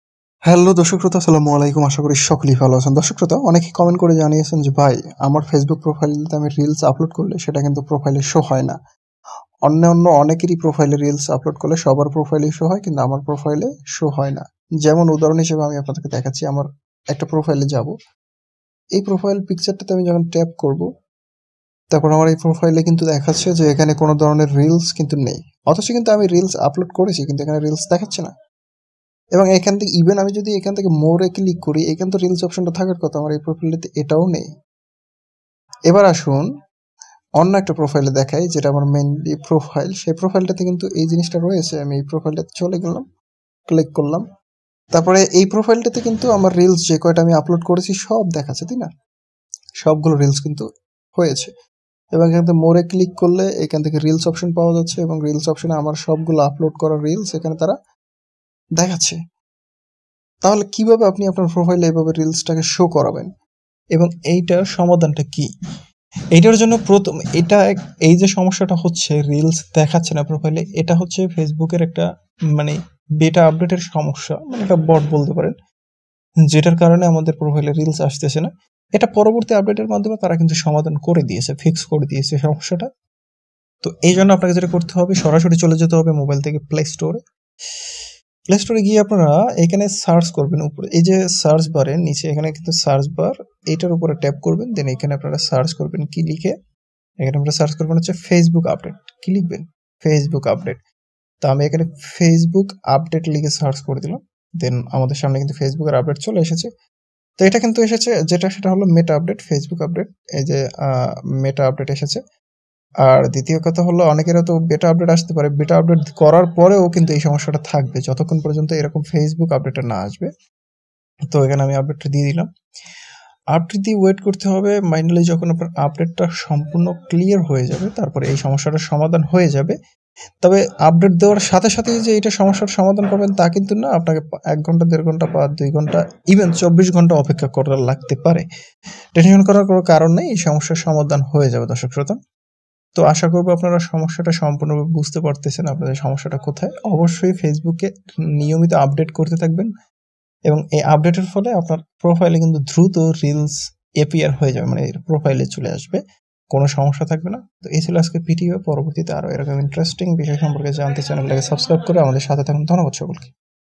reels video show facebook account problem solve | how to fix facebook reels not working
How to post reels video to facebook
By this video you will be able to slove reels video not showing problem on facebook profile